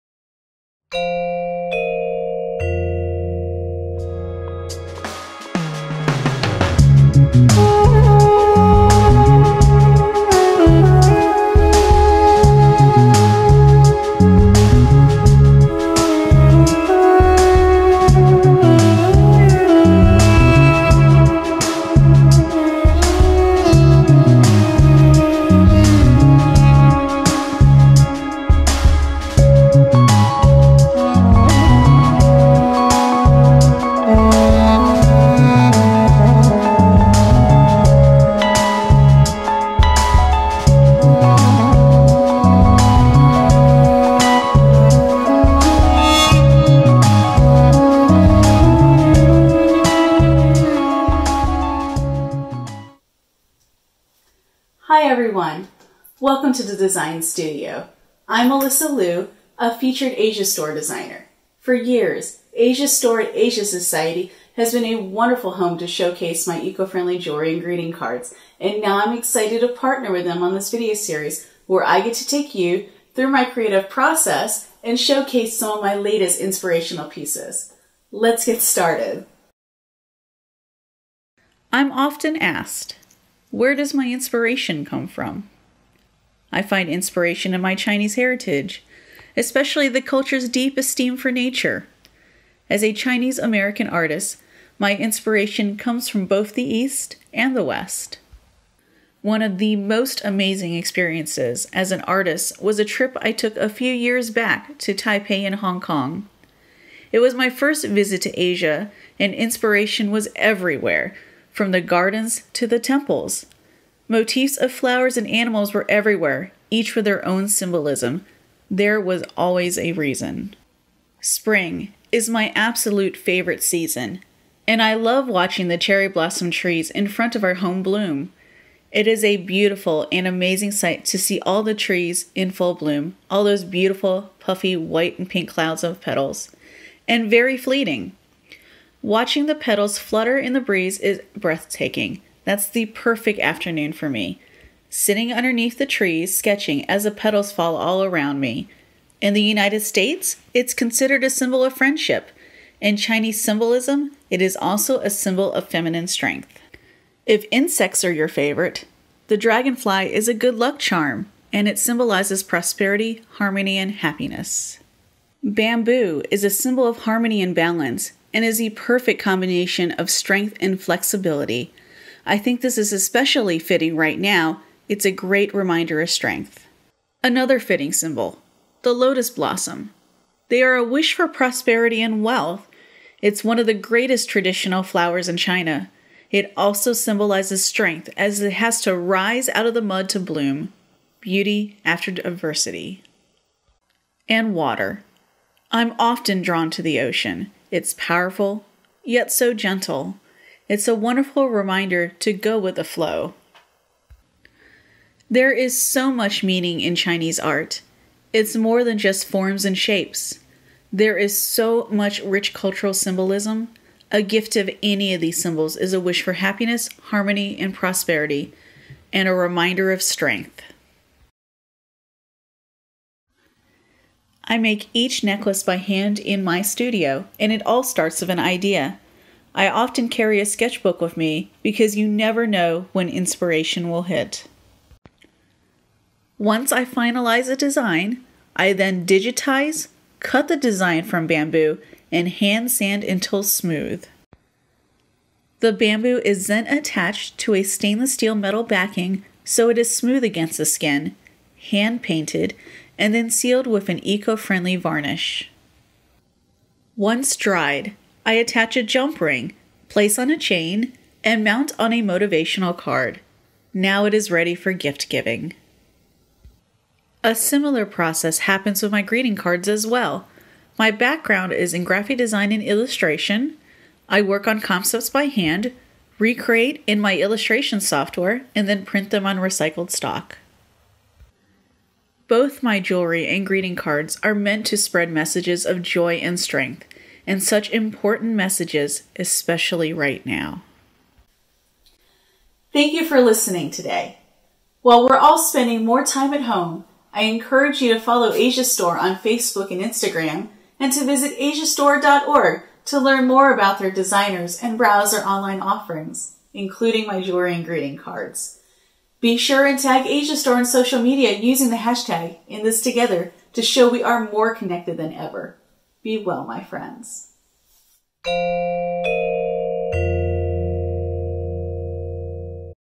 Design Studio: Melissa Lew-Gifts for Inspirational Living
NEW YORK, May 12, 2020 —  Visit Melissa Lew’s Design Studio where she creates modern, inspirational accessories inspired by the wonders of nature, travel, and her Chinese heritage.  (5 min., 22 sec.)

We’re in this together with you. Be InThisTogether with us. Support Asia Society today